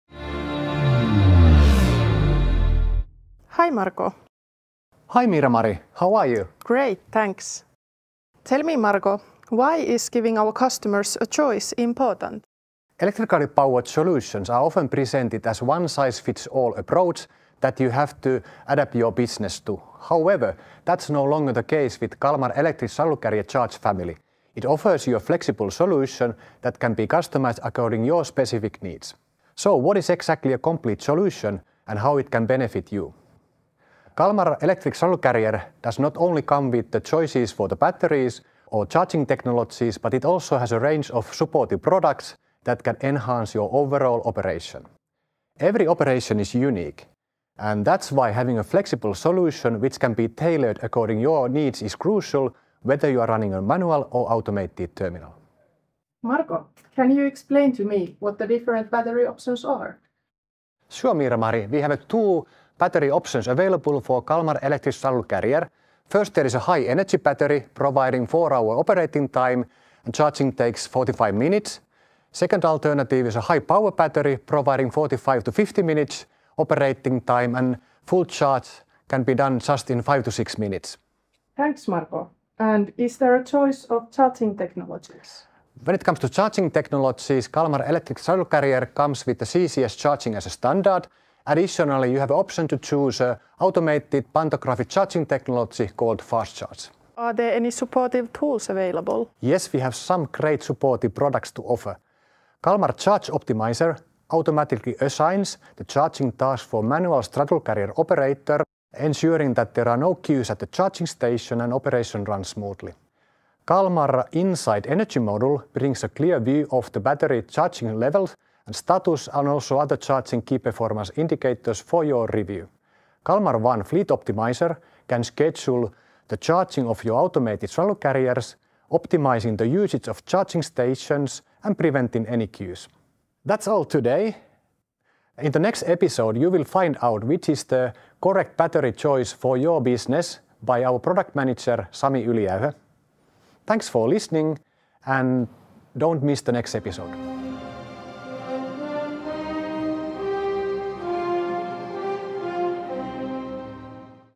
Green Chair - Electric Straddle Carrier Charge Family_Episode 1
Electrically powered solutions are often presented as a one-size-fits-all approach that you have to adapt your business to. However, that is no longer the case with the Kalmar Electric Straddle Carrier Charge Family. It offers a flexible solution that can be customised to meet your specific business needs.
In this Green Chair episode, Marko Hopeaharju, Head of Horizontal Transportation Solutions, Kalmar, will walk you through the Kalmar Electric Straddle Carrier Charge Family.